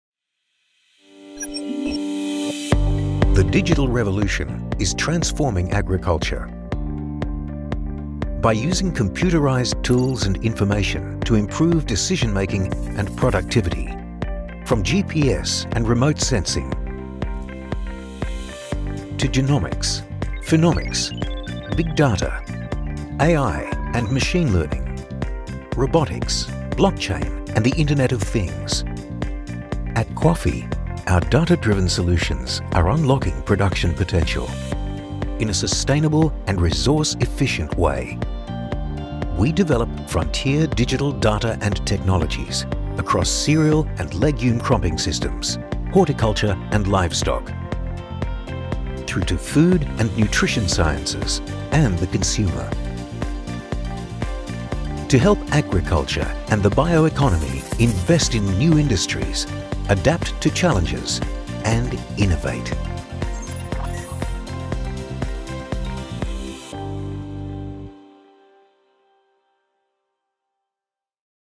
Innovation in Digital Agriculture
The digital revolution continues to transform agriculture, in the way it has transformed other key industries, such as telecommunications, banking and mining.

Digital agriculture makes use of integrated and connected computerised tools and information, to improve decision-making and productivity across all stages of food production – from genetics to farm management, transport and to the consumer.

Agriculture is yet to experience the full effect of digital technology but leads the way in some of the frontier digital sciences, such as linking remote sensing and predictive systems with genetics and genomics. Read more about digital agriculture.